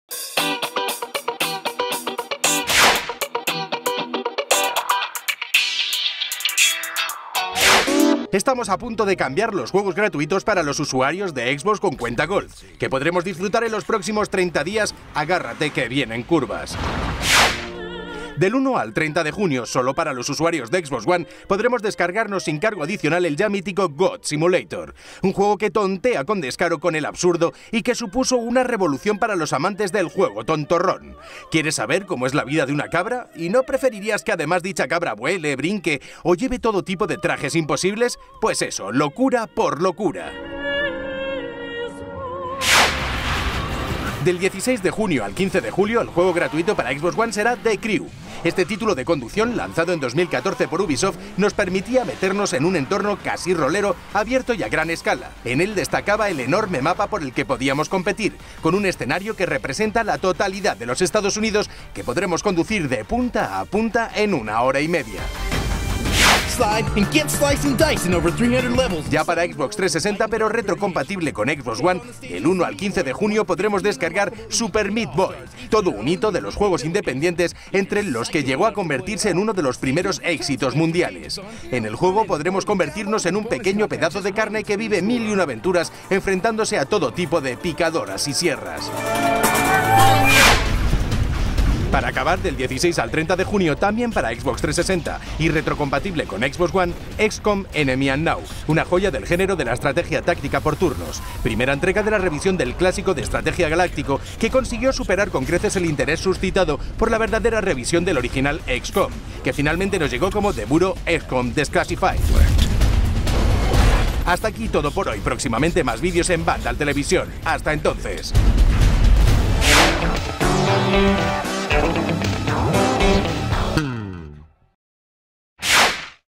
Juegos con Gold de junio 2016
Os contamos qué juegos se podrán descargar este mes los suscriptores de Xbox Live Gold.